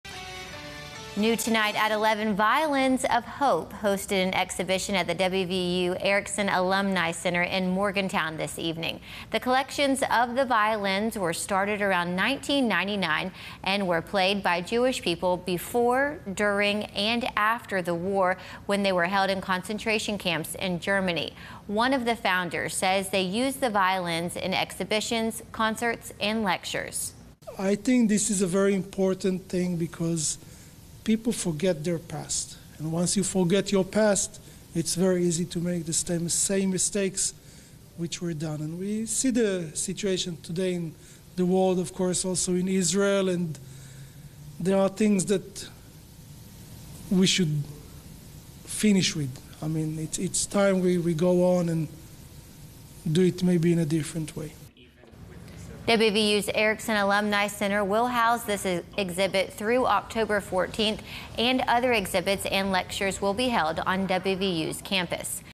Violins of Hope exhibit comes to WVU
An exhibit opened in Morgantown on Tuesday which commemorates the Jewish lives lost and the lives affected by the Holocaust.